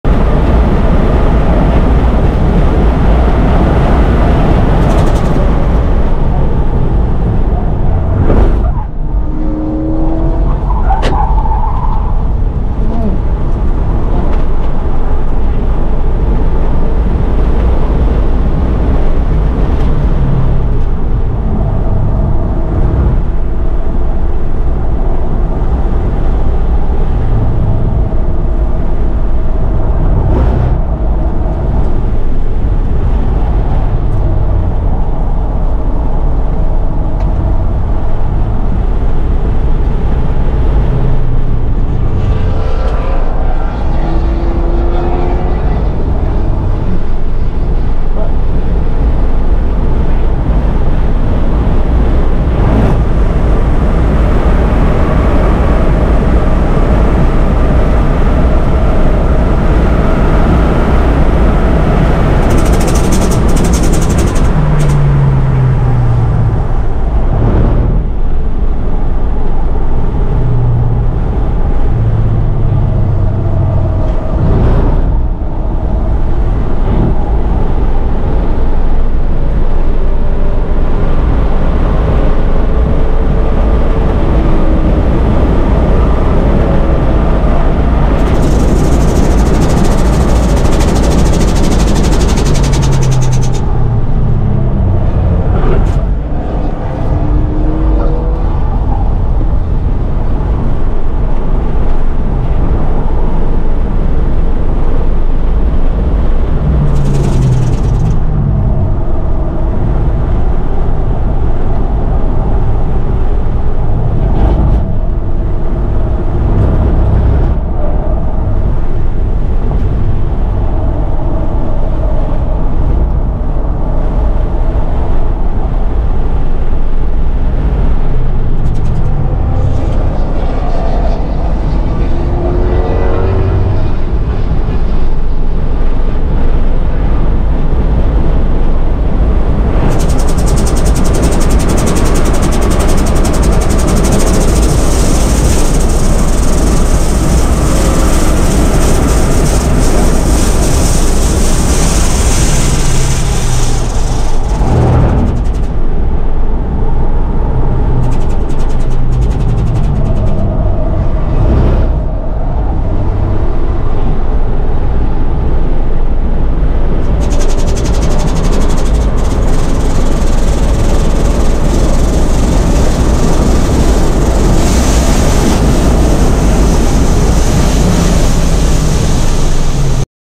Optima USCA PIR 2024 - 2020 Tesla 3P-
2024 Optima Ultimate Street Car Association - Portland International Raceway.
First time here, second TT session (3rd & 4th laps)
126.544 on my first lap ...Splitter ripped from my fascia and was dragging, absolutely horrendous sound! WTF 🙄
new spiller ive got is getting proper mounts!